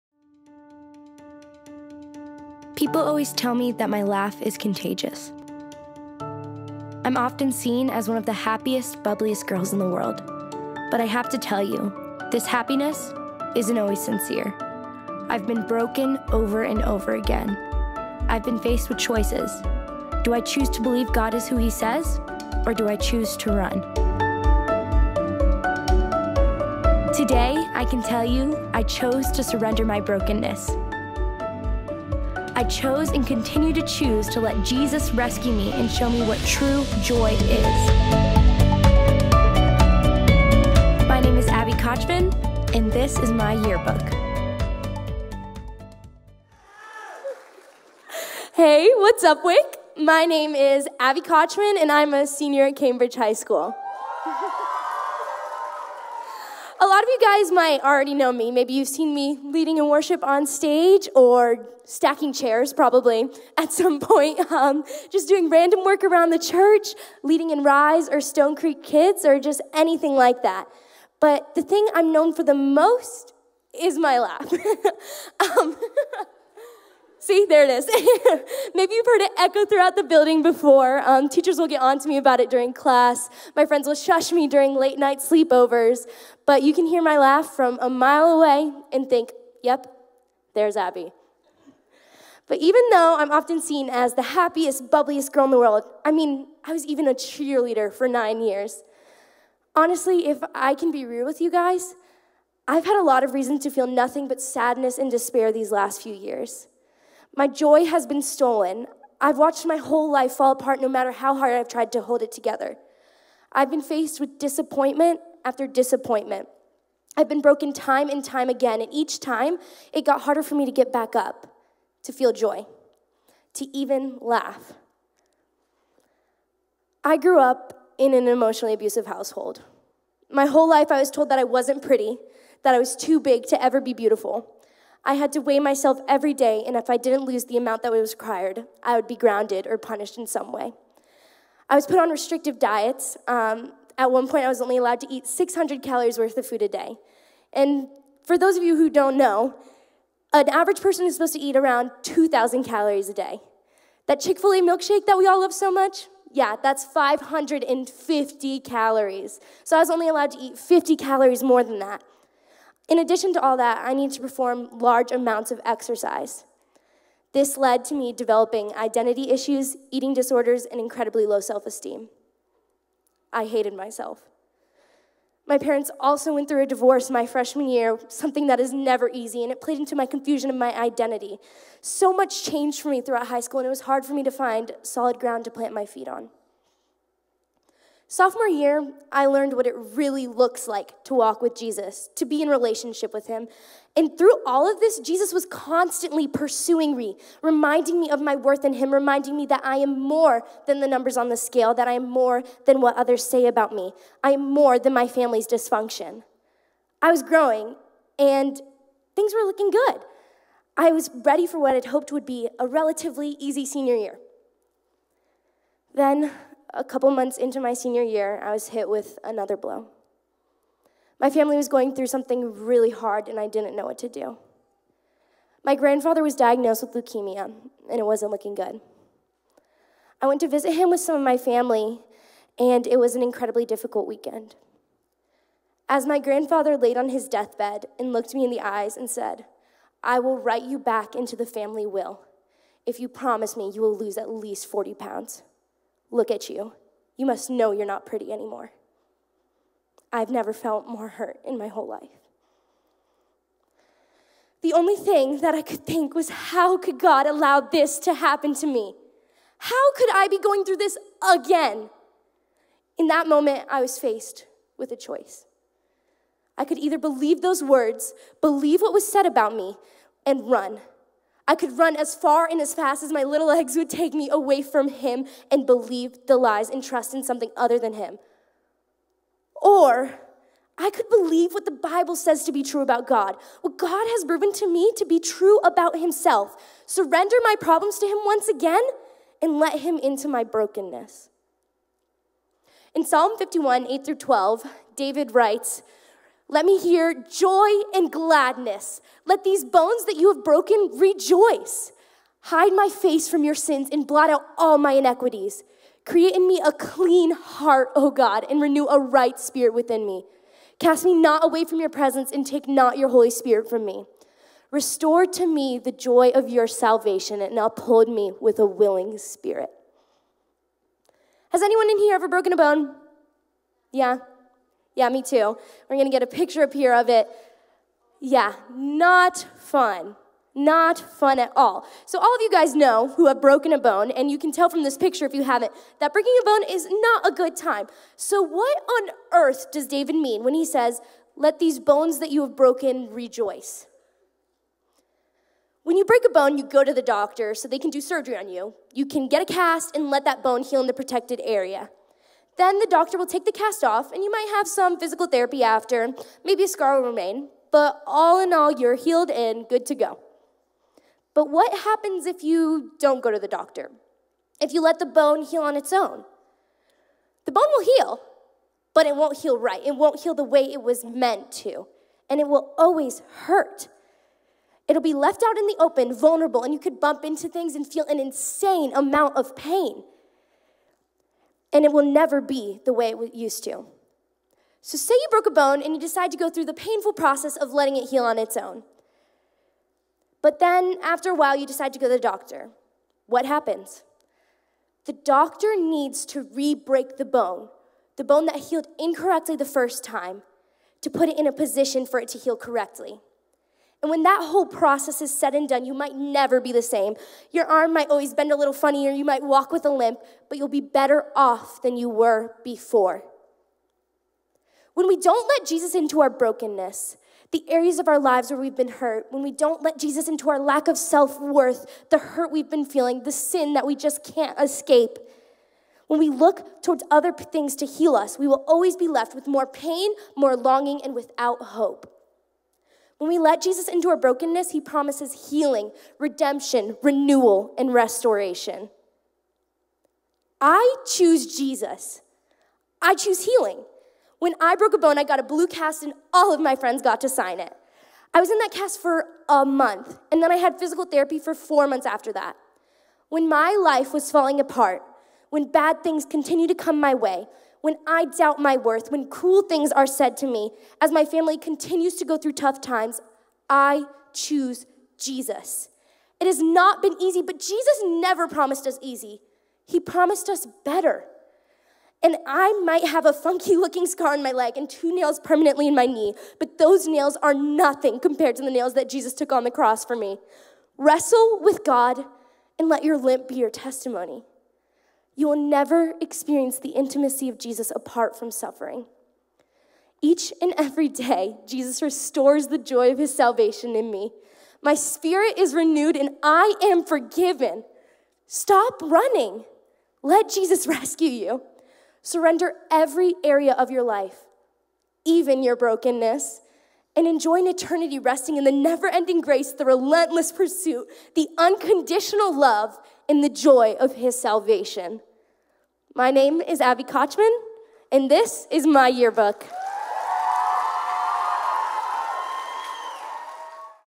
My Yearbook - Abbey Kochman
Jesus transforms lives and rewrites stories! My Yearbook is an opportunity for seniors to share their stories of how Jesus has changed their life and how they have grown in their faith through high school.